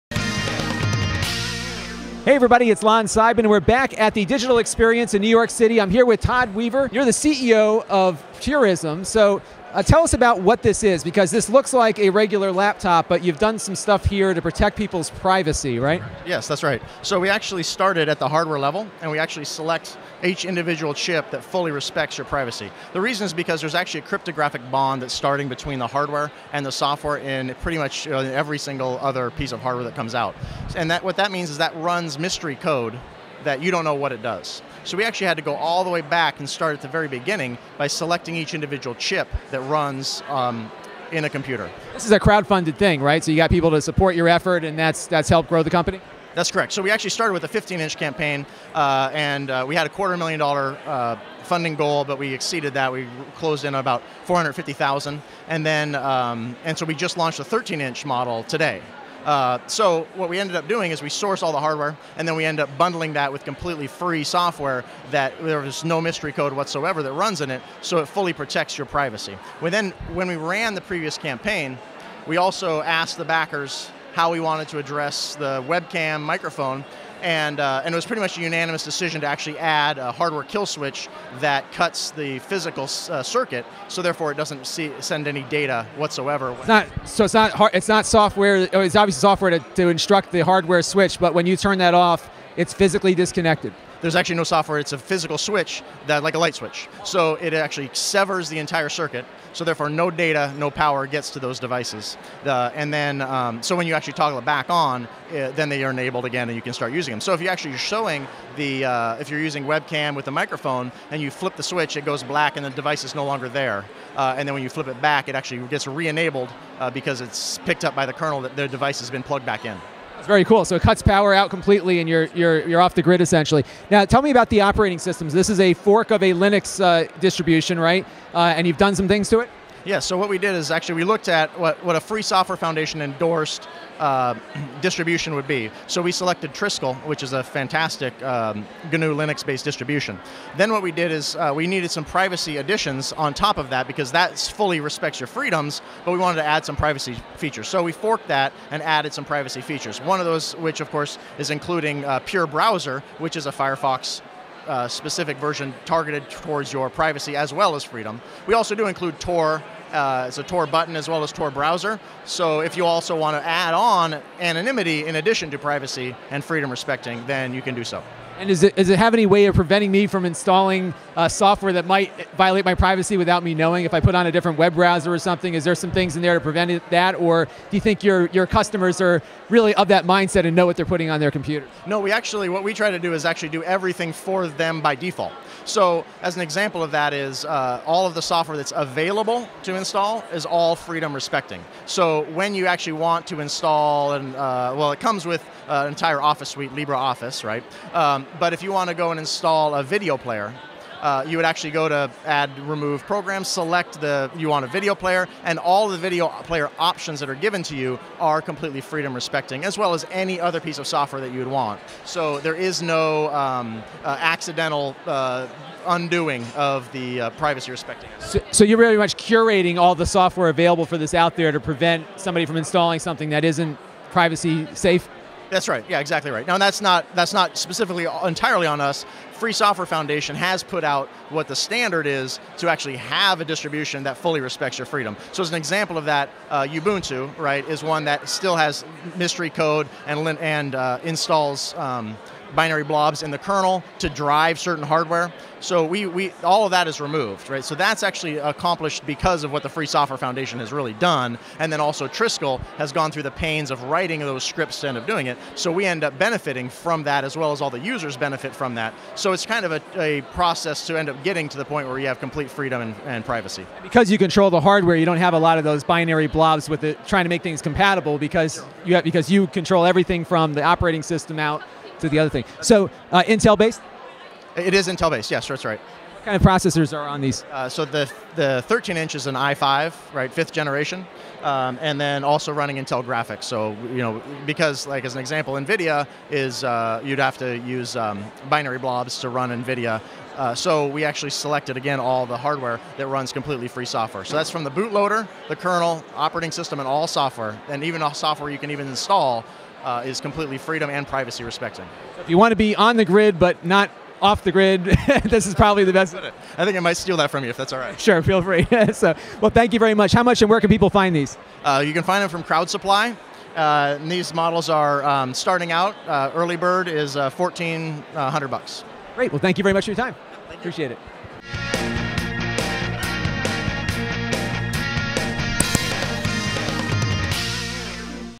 First Look: Purism Privacy Respecting Laptop Computers - Interview with CEO Todd Weaver
Purism develops privacy respecting laptop computers - they source the hardware components and pair it with their own Linux fork designed to completely protect user privacy. It even has a physical switch to disconnect the webcam and microphone.

Because they source all of the hardware components they also have complete control of the drivers that are being used with those computers.

I always love speaking with CEOs of companies as they are often the most knowledgeable about their products. I really enjoyed talking with Todd, he has an interesting concept for PC design here.